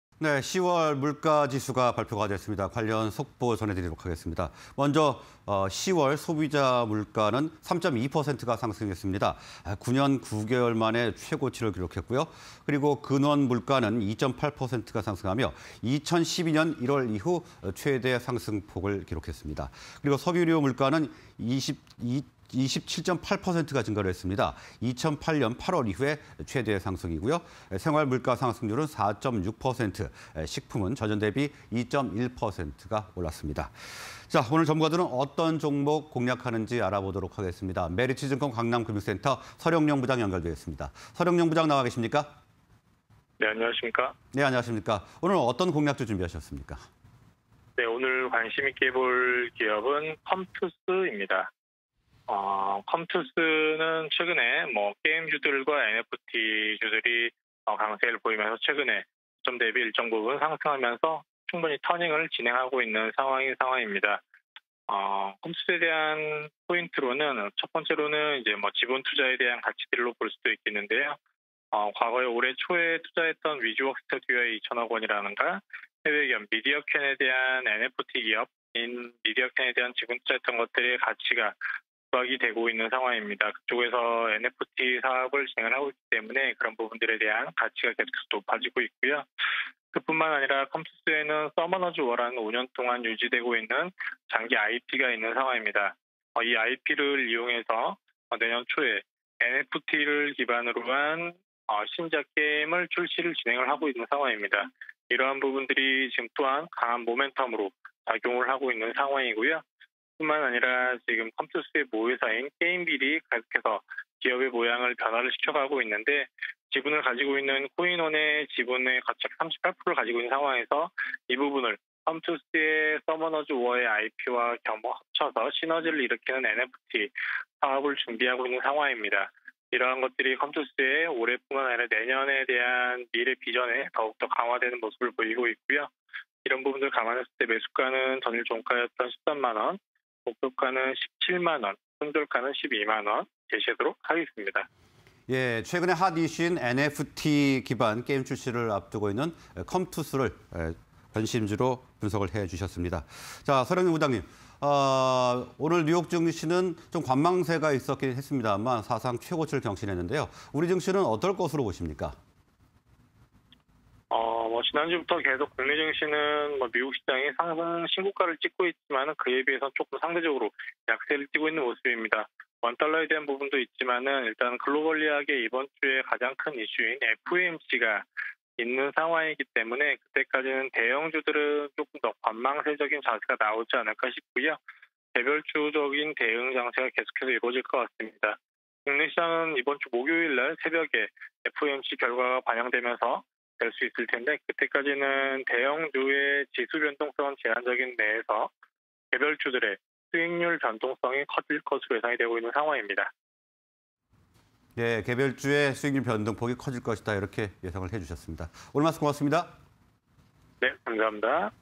컴투스(078340)... 지분 투자 업체 가치 부각_굿머니 공략주 (20211102)
컴투스(078340)
- 지분 투자 업체 가치 부각
- NFT 기반 게임 출시 예정
- 모회사 게임빌의 코인원과 협력 시너지